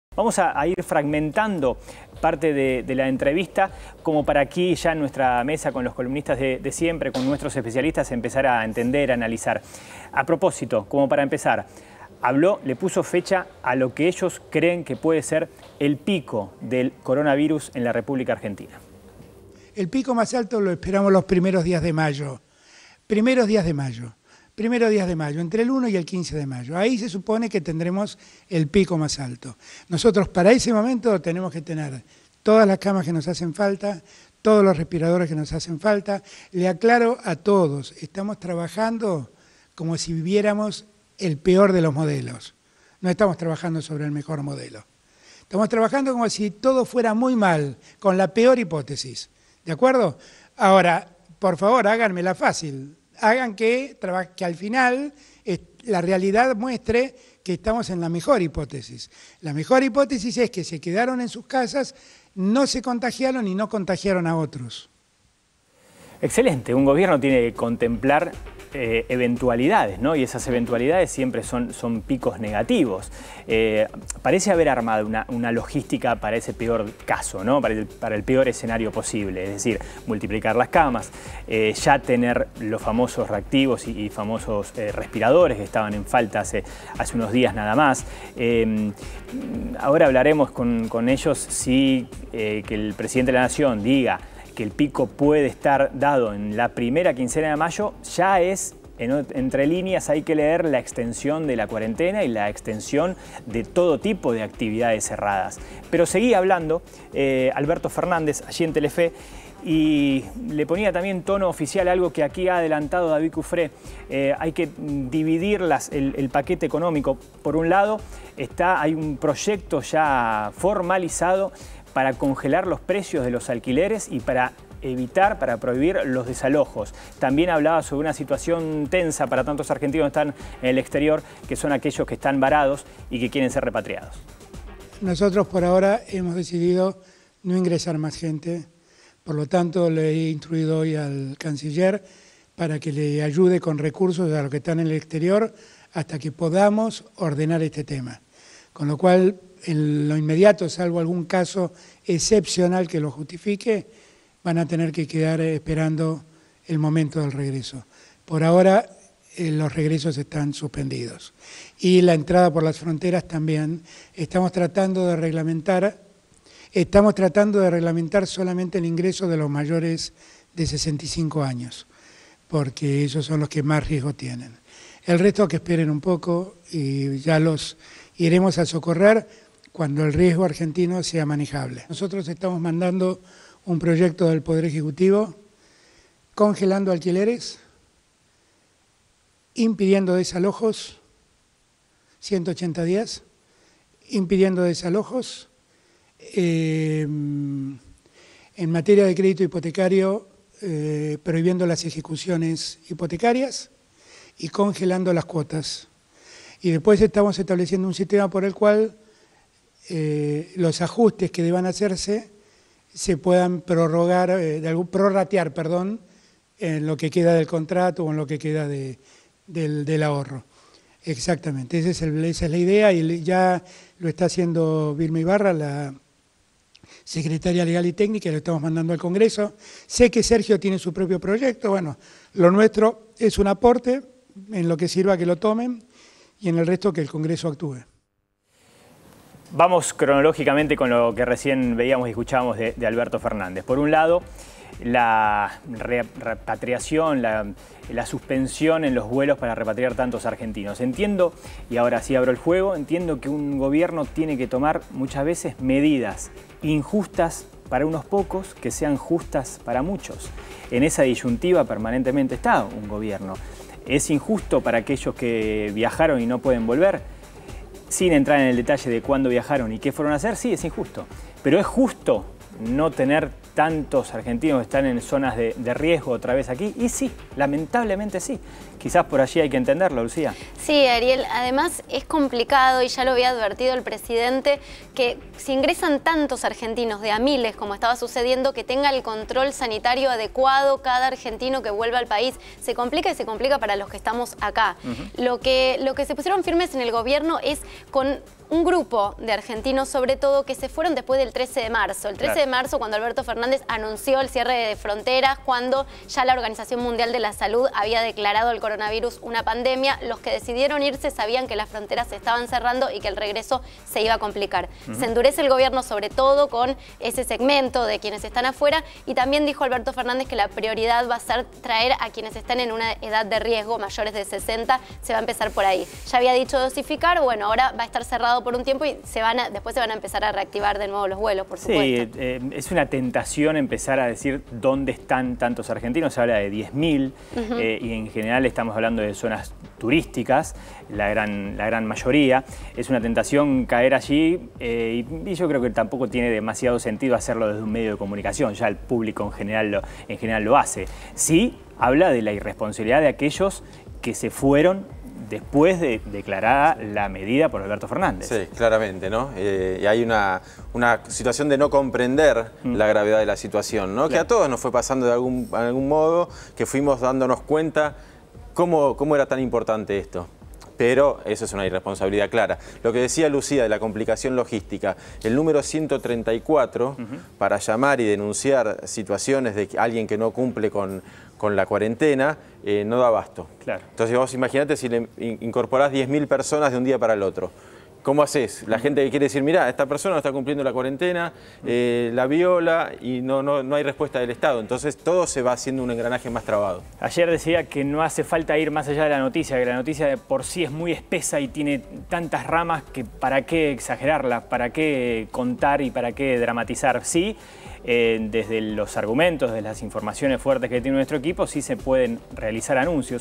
Se detiene la repatriación de argentinos
El presidente Alberto Fernández aseguró este miércoles que "por ahora los regresos" al país "están suspendidos" y que los argentinos varados en el exterior "deberán esperar un poco" para volver al país.